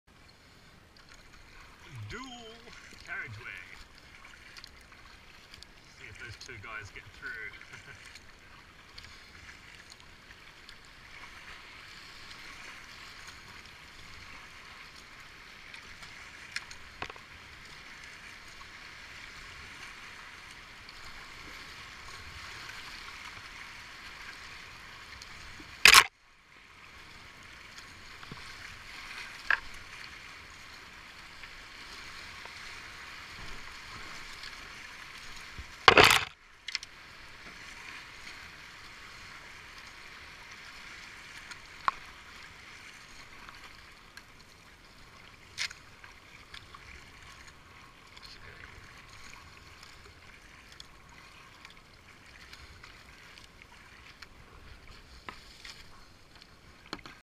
Avon Descent 2016 - 36 Day2 Dual Carriageway
Helmet Camera footage of the 2016 Avon Descent. This one shows Dual Carriageway.

Dual Carriageway is a fast flowing TiTree filled section of river just below Syds Rapids where the river splits into 2-3 channels. The 3rd channel is only open in very high water, and it looks much harder than it is - all of the water is keeping you away from the trees as it's rushing out from under them.. Just don't get your paddle stuck!

The race water level at the Walyunga Paddlers Gauge was 0.72m midday on Sunday and rising rapidly, the calculated TiTrees level was 0.70m (the gauge is now inaccurate, we use instead)